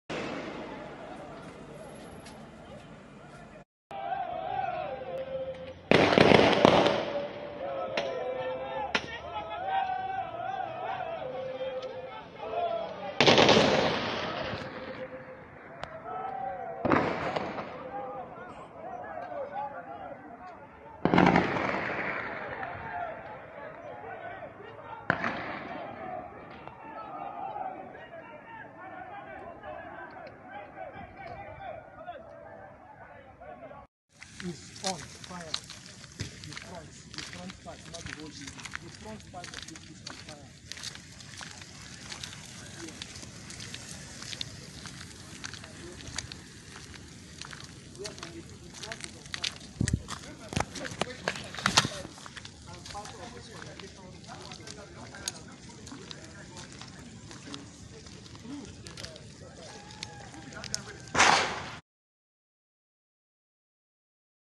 Owerrijailbreak. Prisons on fire. Owerri is hot😭🔥‍‍‍‍‍‍🔥‍‍‍‍‍‍🔥‍‍‍‍‍‍🙆‼️
Owerri is hot now. Owerriprisonbreak Owerrijailbreak is happening. As you stay careful and stay home, Join the TMIT8.0 to kill the fear and boredom.